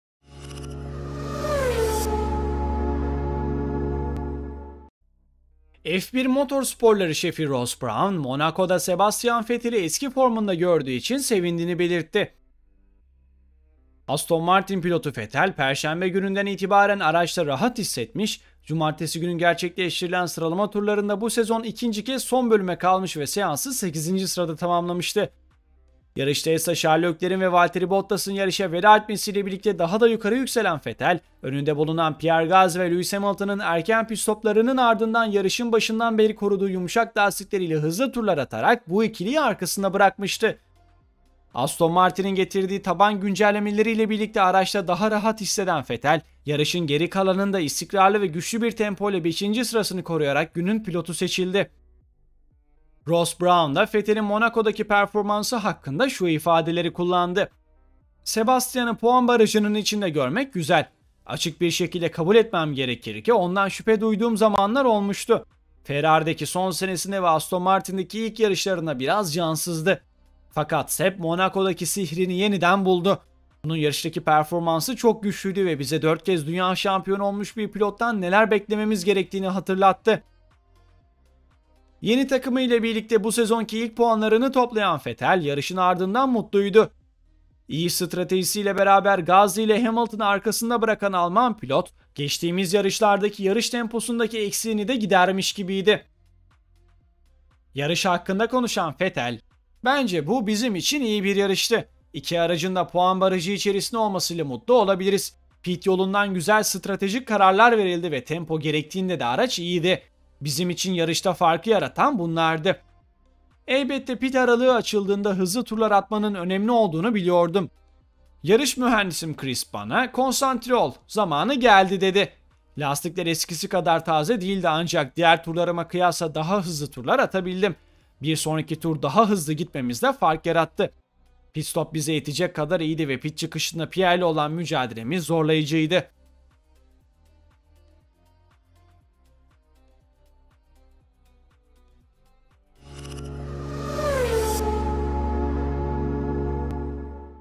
''Vettel, Monako'da sihrini yeniden buldu''
Aston Martin pilotu Vettel, perşembe gününden itibaren araçta rahat hissetmiş, cumartesi günü gerçekleştirilen sıralama turlarında bu sezon ikinci kez son bölüme kalmış ve seansı 8. sırada tamamlamıştı.

Yarışta ise Charles Leclerc'in ve Valtteri Bottas'ın yarışa veda etmesiyle birlikte daha da yukarı yükselen Vettel, önünde bulunan Pierre Gasly ve Lewis Hamilton'ın erken pit stoplarının ardından yarışın başından beri koruduğu yumuşak lastikleriyle hızlı turlar atarak, bu ikiliyi arkasında bırakmıştı.

Aston Martin'in getirdiği taban güncellemeleriyle birlikte araçta daha rahat hisseden Vettel, yarışın geri kalanında istikrarlı ve güçlü bir tempoyla 5. sırasını koruyarak günü pilotu seçildi.

Bu ve bunun gibi daha fazla içeriğe ulaşmak için sitemizi ziyaret edebilirsiniz

Ayrıca Discord kanalımıza katılarak sizler de eğlenceye ortak olabilirsiniz!